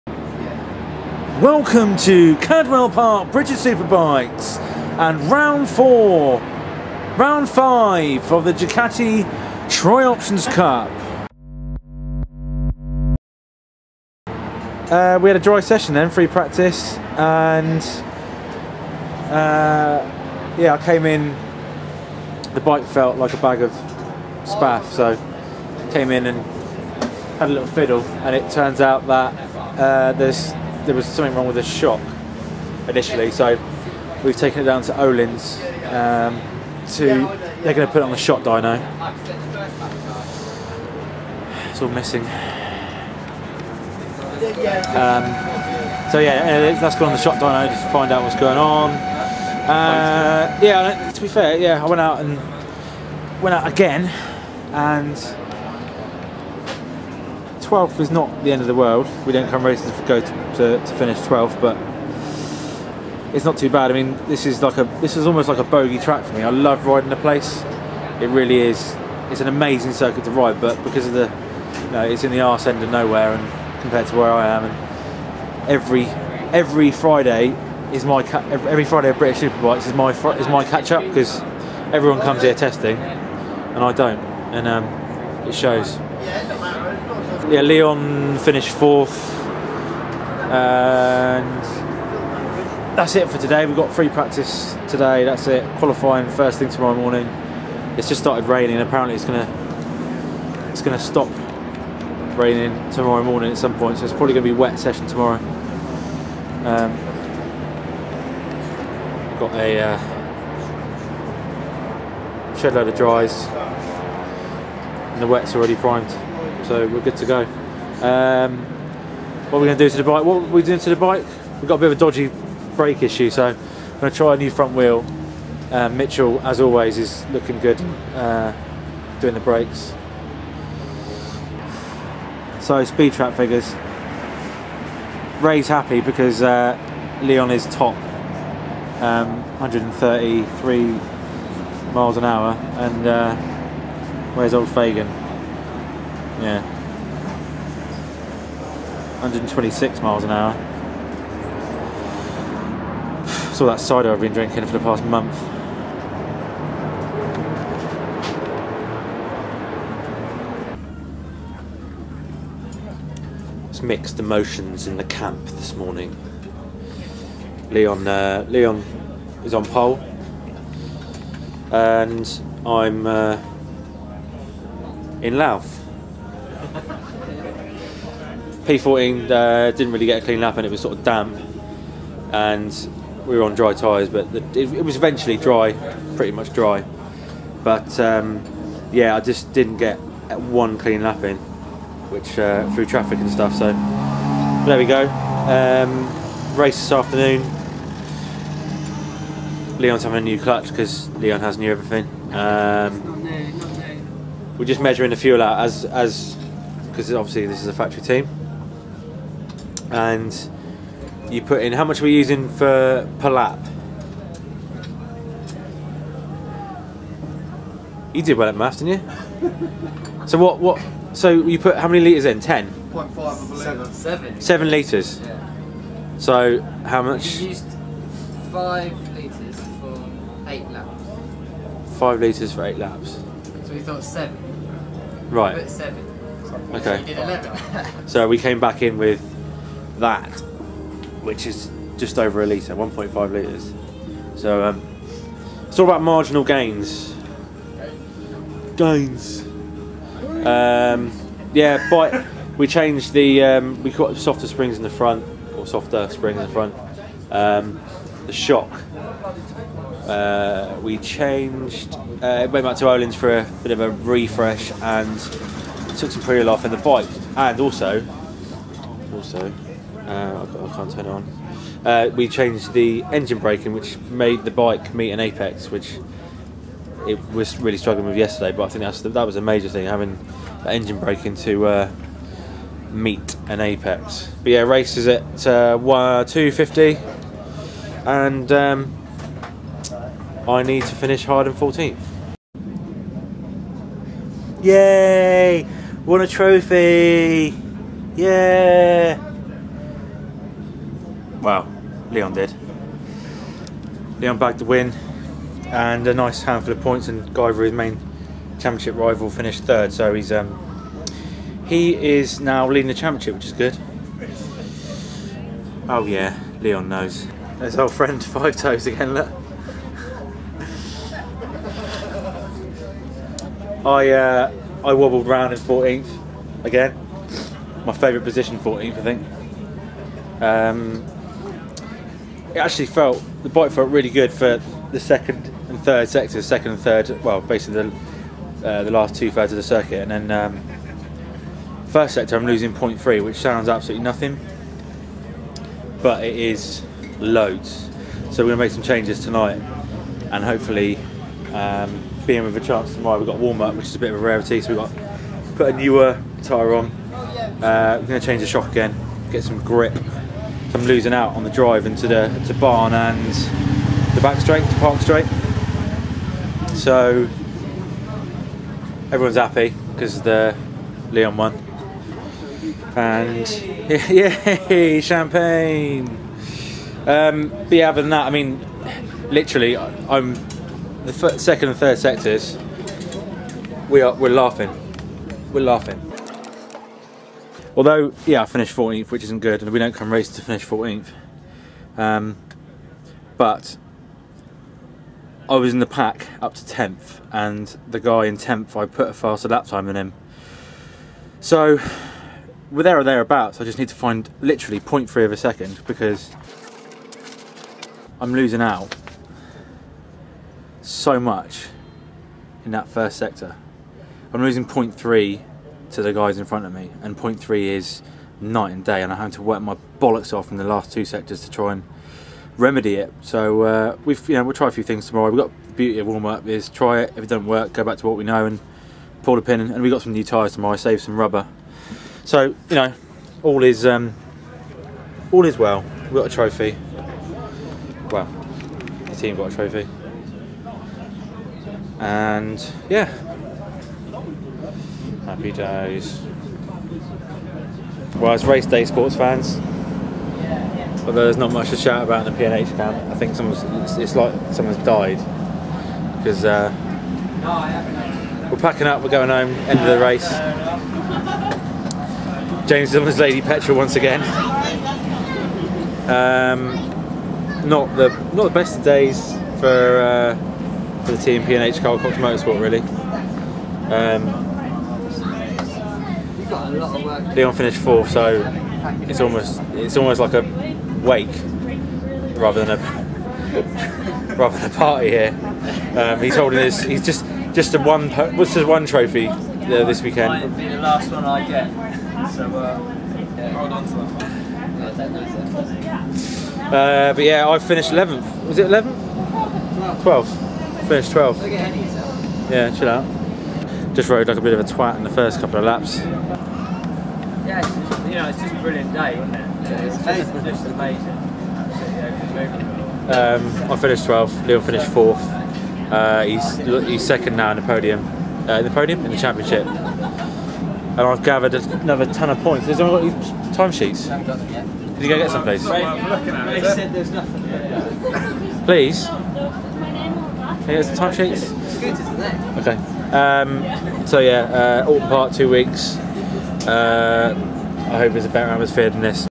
2016 Ducati Cup: Cadwell Park
Apologies for the lack of enthusiasm in this edit. Hard to go racing when your team-mate is winning races and you're finishing 12th. Cadwell has always been a bogey...